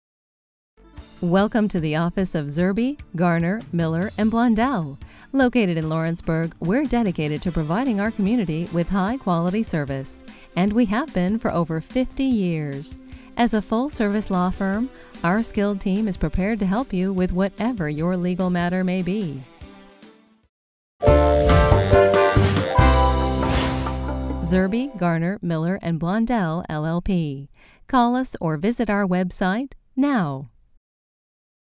Zerbe Garner Miller and Blondell LLP
Welcome to the office of Zerbe Garner Miller and Blondell!
Located in Lawrenceburg, were dedicated to providing our
community with high quality service, and we have been for
over 50 years. As a full service law firm, our skilled team
is prepared to help you with whatever your legal matter may be.